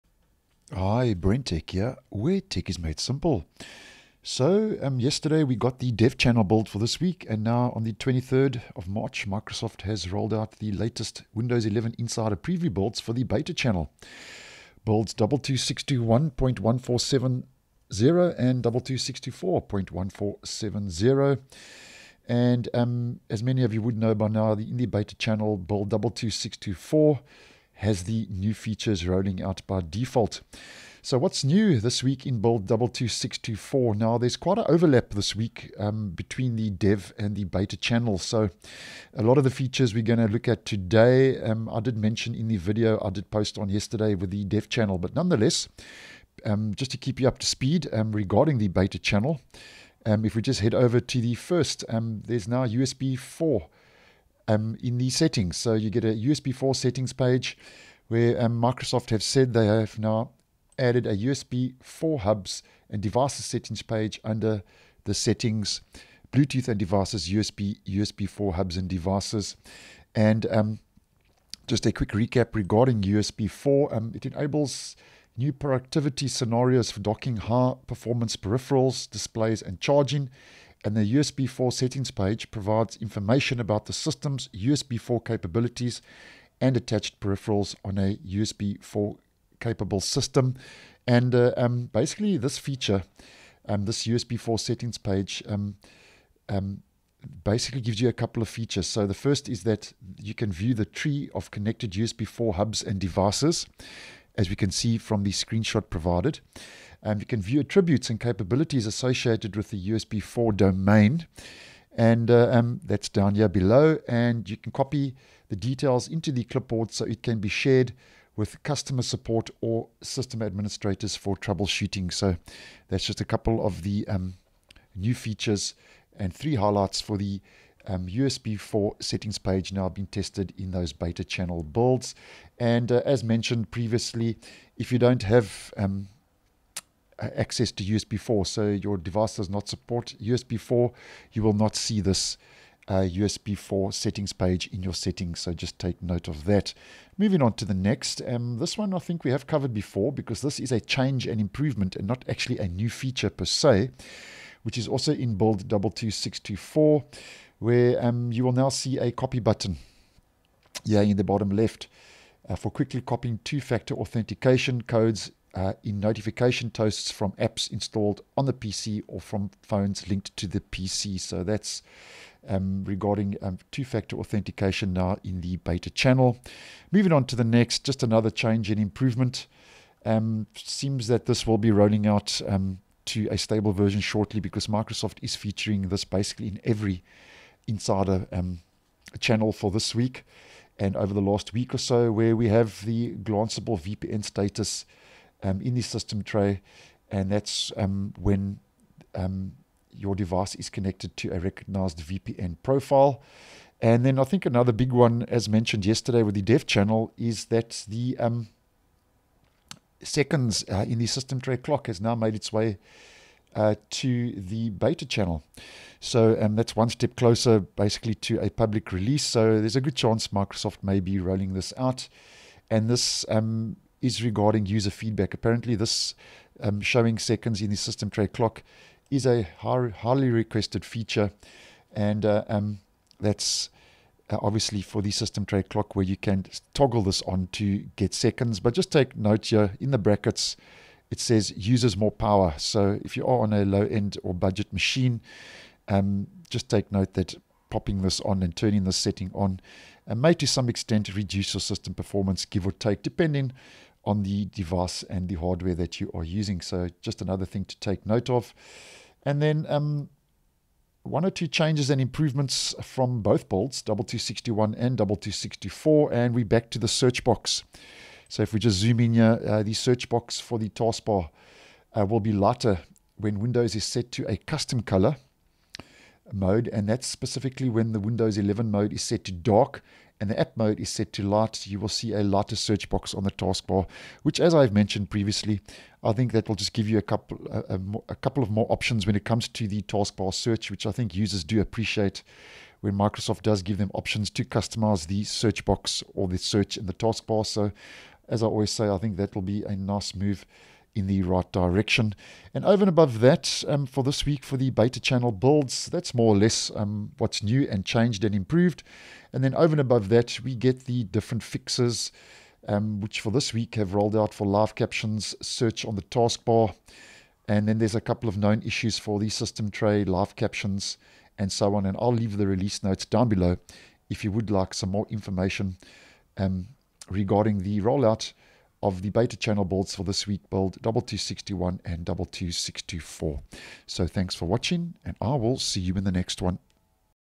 Windows 11 Builds 22621 & 22624.1470: New USB4 page, seconds in system tray clock and more
Microsoft are rolling out a pair of builds to Insiders in the Beta Channel this week. Windows 11 Insider Preview Build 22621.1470 and Build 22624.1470 (KB5023780), that also includes, two-factor authentication improvements, glanceable VPN status in the system tray, the search box on taskbar will be lighter when Windows is set to a custom color mode, changes and improvements